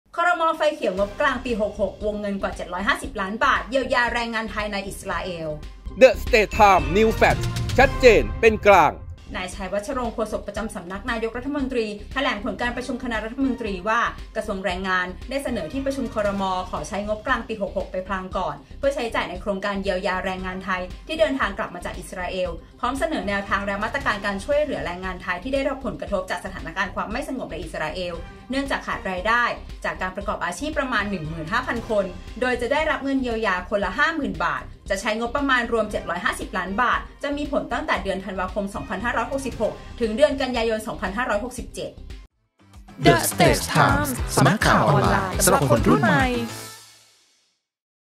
ครม.ไฟเขียวงบกลางปี 66 เยียวยาแรงงานไทยในอิสราเอล | NewsFlash 13-12-23 | 13.00 น.
NewsFlash ประจำวันที่ 13 ธันวาคม 2566
ครม.ไฟเขียวงบกลางปี 66 วงเงินกว่า 750 ล้านบาท เยียวยาแรงงานไทยในอิสราเอล
นายชัย วัชรงค์ โฆษกประจำสำนักนายกรัฐมนตรี แถลงผลการประชุมคณะรัฐมนตรี (ครม.) ว่า กระทรวงแรงงาน ได้เสนอที่ประชุม ครม.ขอใช้งบกลางปี’66 ไปพลางก่อน  เพื่อใช้จ่ายในโครงการเยียวยาแรงงานไทยที่เดินทางกลับมาจากอิสราเอล พร้อมเสนอแนวทาง และมาตรการการช่วยเหลือแรงงานไทยที่ได้รับผลกระทบจากสถานการณ์ความไม่สงบในอิสราเอล